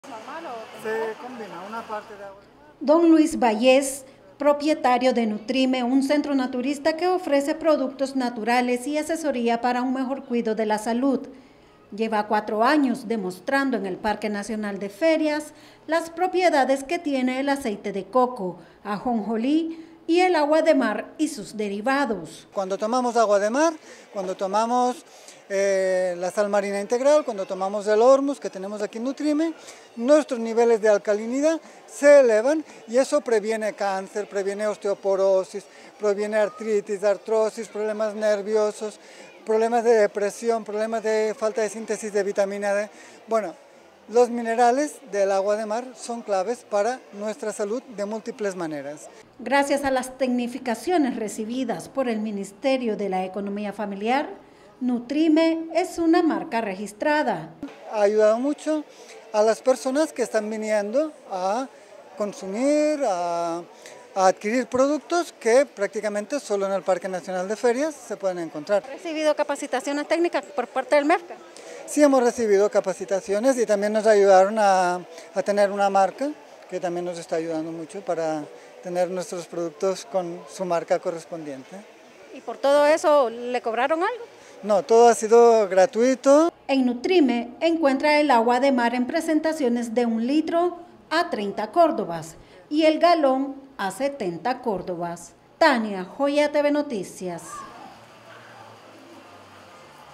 NUTRIME, LLEVANDO PRODUCTOS NATURALES A LAS FAMILIAS NICARAGÜENSES
Don Luis Vallés propietario de Nutrime, un centro naturista que ofrece productos naturales y asesoría para un mejor cuido de la salud, lleva 4 años demostrando en el parque nacional de ferias las propiedades que tiene el aceite de coco, de ajonjolí y el agua de mar y sus derivados.